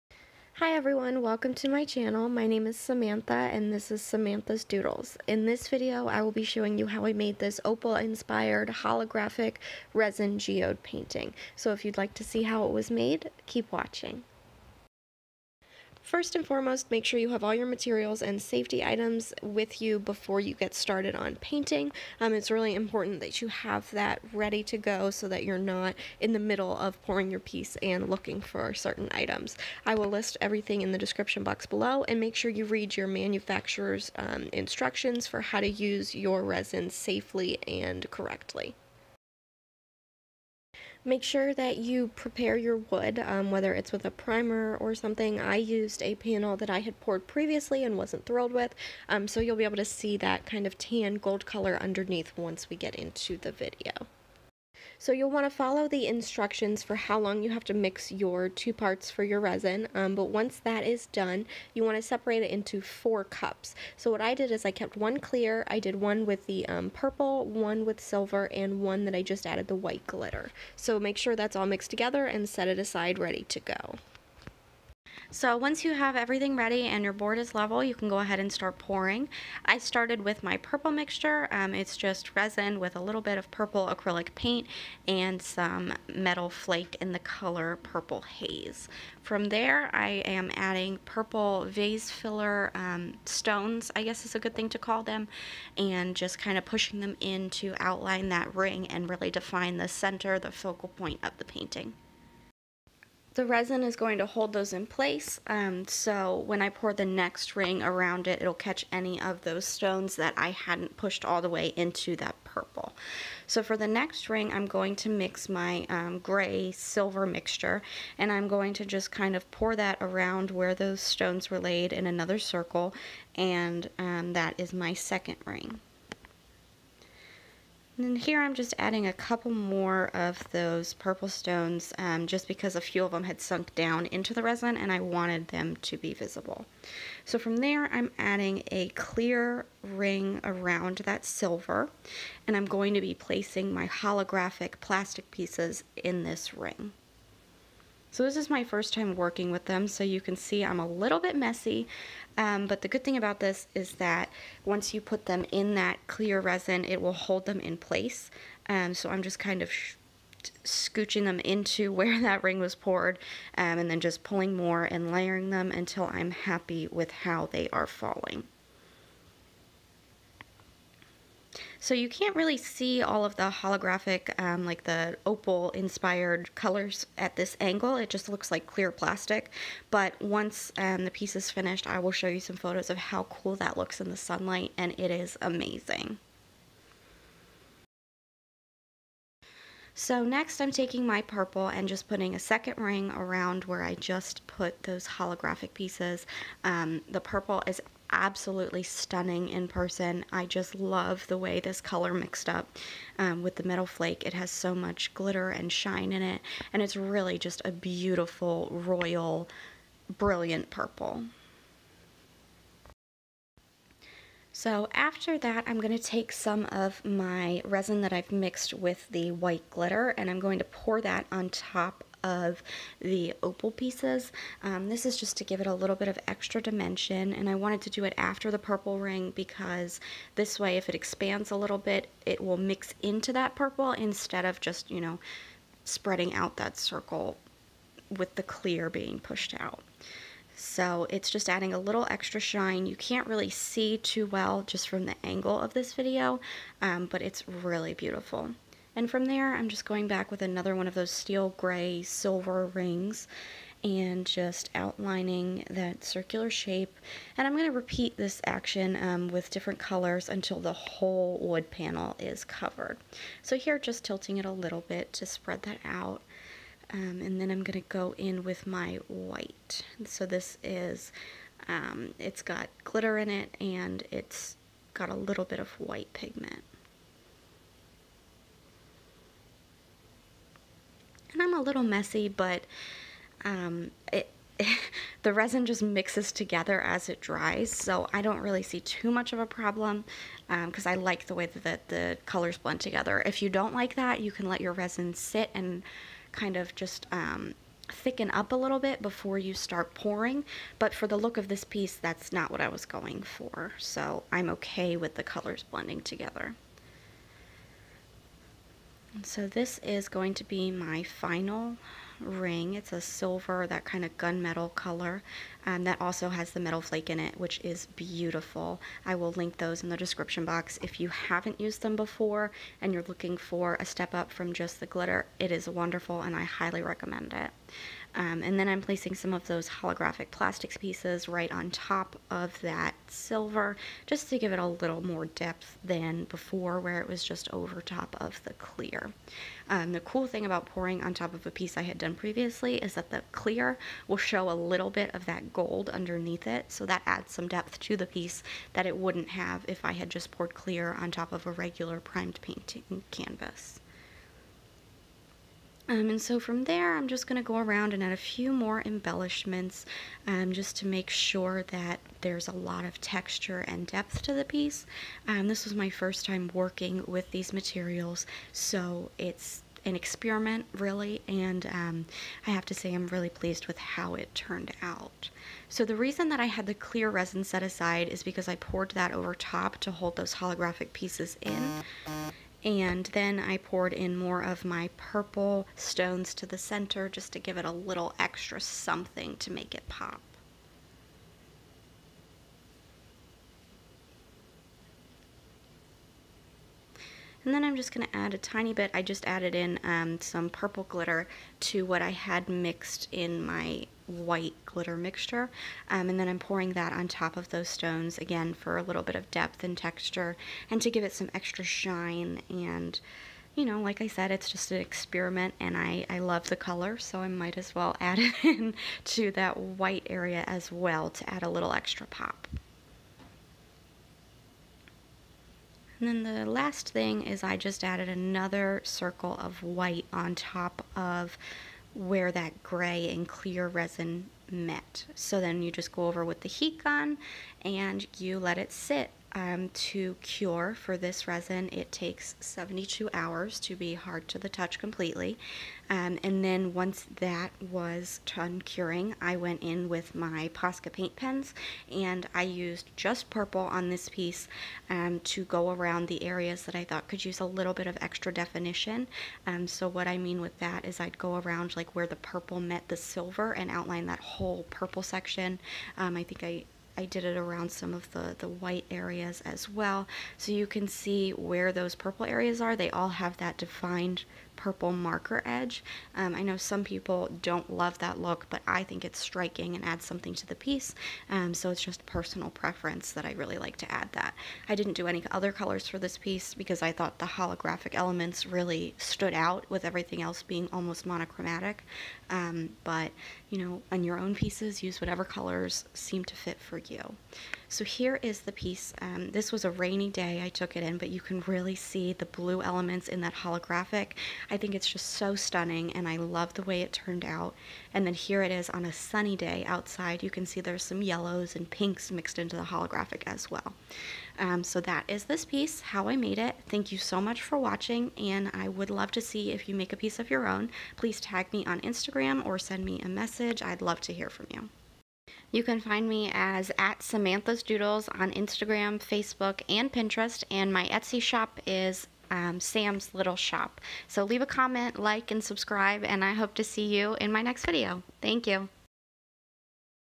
Holographic Purple Resin Geode Opal Experiment by SamanthasDoodles
This video is my take on the holographic opal trend that was  popularized by Crystal Ma. I put my own spin on it by incorporating the technique into one of my geode paintings. For this attempt I didn't use any silicone oil even though it's been a staple of my past few tutorials. The rest of the materials will be listed below.

I mixed the materials off camera, but used Art Resin (a 1:1 ratio, stir for 3 minutes) and mixed in acrylic paint (less than 10% of color to resin) and other additives.  Check out my Pink Resin Geode Tutorial for a full visual explanation of the resin mixing process.

I am on Instagram, Pinterest, and Facebook as @SamanthasDoodles 💗

Acrylic Paint (parchment, purple and silver)

Measuring Cups

Craft Glitter (white)

Posca paint pens

Purple vase filler from Michaels

Holographic film pieces from Michael's - can't find the exact link but looks like this cut up:
Pacon Iridescent Film, Mother of Pearl, 36" x 12-1/2', 1 Roll